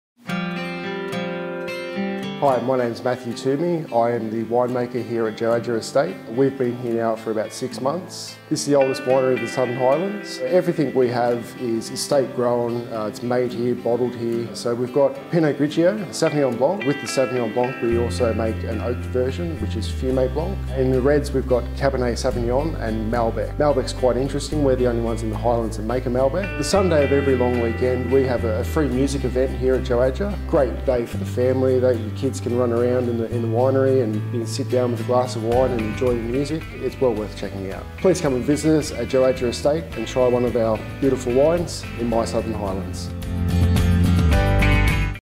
Meet the Wine Makers Joadja Estate
Meet Matt from Joadja Estate, learn about the wonderful range of wines.